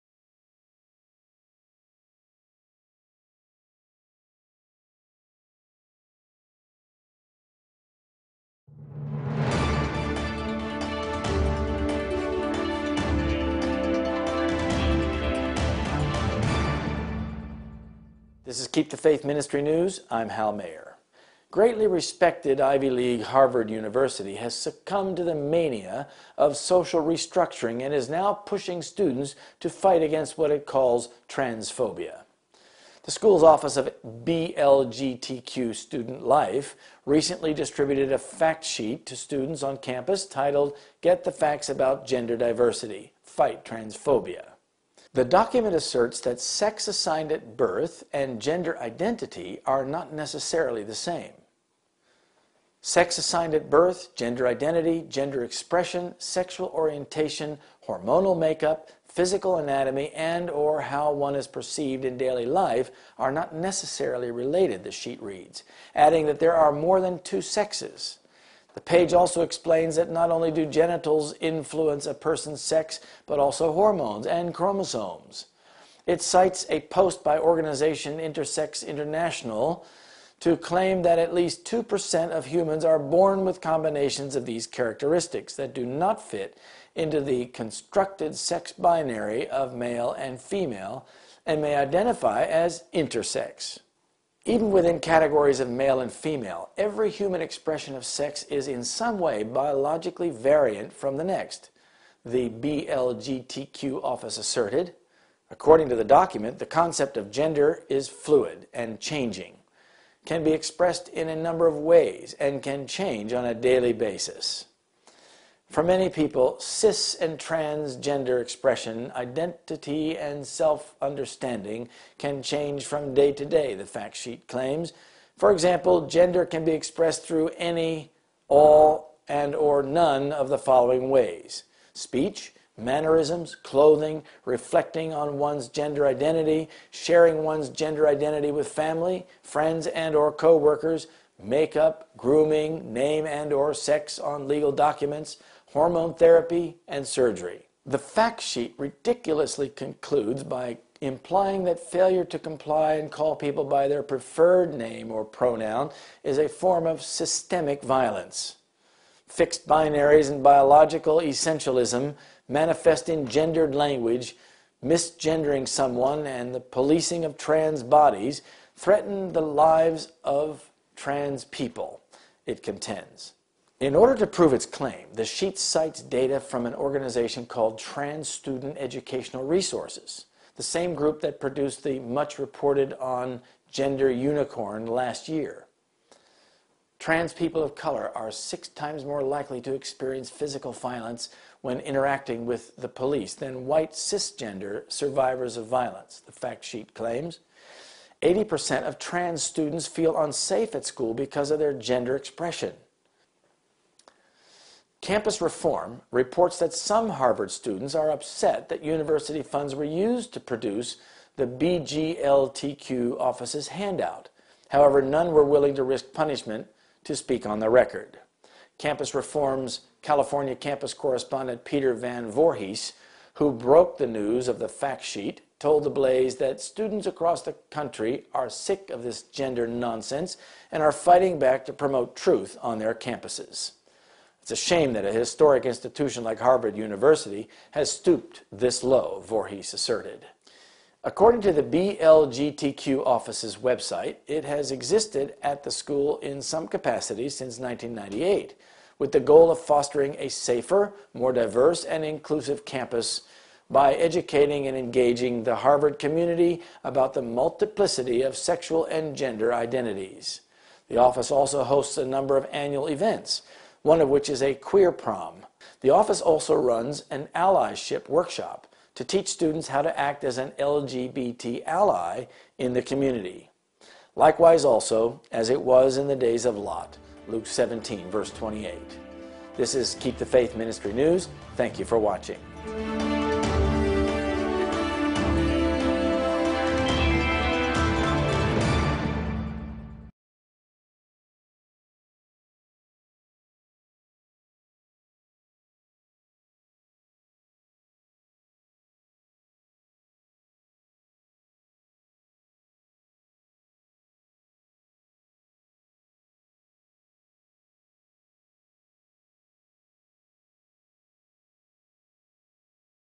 [세계뉴스] - LGBTQ (동성애, 양성애, 트랜스젠더, 퀴어) 라이프스타일을 지지하는 아이비리그 명문 하버드
아이비리그 명문 하버드 대학은 사회 재구성 마니아에 굴복하여
이제 학생들에게 그들이 "트랜스포비아"라 부르는 것과 싸울 것을 촉구하고 있습니다.
대학의 BLGTQ 학교생활 사무소는 최근 "성 다양성의 팩트를 이해하고 트랜스포비아에 대항하라."는 제목의 팩트시트(factsheet)를 캠퍼스의 학생들에게 배포했습니다.
이 문서는 "타고난 성별과 개인의 성별 인식은 반드시 같지 않다."고 주장했습니다.
(팩트시트) "타고난 성별과 성별 인식과 성별의 표출과 성적 지향과 호르몬 구성과 해부학적 구조와
한 개인이 매일 어떻게 여겨지길 바라는 성별은 반드시 연관성이 있는 것은 아니다.
두 성별보다 더 많은 성별이 있다."
이 문서는 생식기 외에도 호르몬과 염색체가 한 사람의 성별에 영향이 있다고 설명했습니다.
인터섹스 인터내셔널이란 단체의 주장을 인용하며
최소 2%의 사람은 "남성과 여성이란 두 분류의 성별에 속하지 못하고 간성(間性)으로 분류할 수 있다."고 주장합니다.
"심지어 남성과 여성이란 범주 안에도, 모든 사람의 성별의 표현은 다른 사람들과 생물학적으로 서로 다르다."고 BLGTQ 사무소는 주장했습니다.
이 문서에 의하면, 성별이라는 개념은 "유동적이며 변하며" 매일 다양한 형태로 표현되며 변할 수 있답니다.
(팩트시트) "많은 사람에게 시스젠더와 트랜스젠더의 표출과 정체성과 자기이해는 매일 변할 수 있다.
예를 들어, 성별은 다음 방식으로 혹은 다음 방식 이외로도 표현될 수 있다:
말과 매너리즘과 옷차림에 성 정체성을 반사하고,
가족들과 친구들과 동료들에게 성 정체성을 공개하고,
화장, 몸단장, 법적 문서에 명시된 이름과 성별,
호르몬 치료, (성전환) 수술."
이 팩트시트는 말도 안 되게 결론을 내리길, 사람들을 그들이 원하는 이름으로 부르지 않거나
그들의 성 정체성을 부정하는 일은 "체계적인 폭력의 형태이다."
(팩트시트) "두 성별로 고정된 생물학적 본질주의, 언어로 표현하는 성별,
누군가의 성 정체성을 잘못 부르는 행위, 트랜스젠더들에게 (생물학적 성에 따른) 규칙 준수를 요구하는 일,
이런 일들은 트랜스젠더들의 삶을 위협한다."
그들의 주장을 증명하기 위해 이 팩트시트는 "트랜스젠더 학생 교육 자원"이라는 단체의 자료를 인용합니다.
작년에 많이 보도된 "젠더 유니콘"을 만들어낸 그 동일한 단체 말입니다.
(팩트시트) "경찰과 맞닥뜨렸을 때, 트랜스젠더 유색인종은 백인 시스젠더에 비해 여섯 배나 육체적 폭력을 경험할 가능성이 있다."
80%의 "트랜스젠더 학생들은 그들의 성 정체성의 표현 때문에 학교에서 안전하지 않다고 느낀다."
어떤 하버드대 학생들은 대학의 자금이 BGLTQ 사무소의 배포물을 만드는데 사용된 점에 불만을 느낀다고 캠퍼스 리폼은 보도했습니다.
그러나 처벌을 두려워했기에 그 누구도 실명을 밝히길 원치 않았습니다.
팩스시트에 관한 기사를 쓴 캠퍼스 리폼의 캘리포니아 캠퍼스 기자 피터 밴 보올히스는 더블레이즈 언론사에 말하길,
"전국의 학생들은 이 성별 난센스에 지쳤고, 그들은 그들의 캠퍼스에서 진실을 알리기 위해 맞대응하고 있다.
하버드 대학과 같이 역사적인 기관이 이 정도로 낮게 떨어졌다는 점은 부끄러운 일이다."
BLGTQ 사무소의 웹사이트에 의하면, 그들은 학교에 1998년부터 조금 규모를 가지고 존재해왔는데,
그들의 목표는 "더 안전하고 더 포괄적이며 포용적인 캠퍼스를
성과 성 정체성의 다양성에 대해 하버드 커뮤니티와 관계를 맺으며 교육함으로 이루는 것"이라합니다.
이 사무소는 매년 몇 번의 이벤트를 주최하는데, 그 중에는 "퀴어 무도회"가 있습니다.
또한 "(퀴어) 동맹 워크숍"을 주최하며 학생들이 어떻게 커뮤니티에서 LGBT를 지원할 수 있는지 가르칩니다.

(눅 17:28) "또 롯의 때와 같으리니 ..."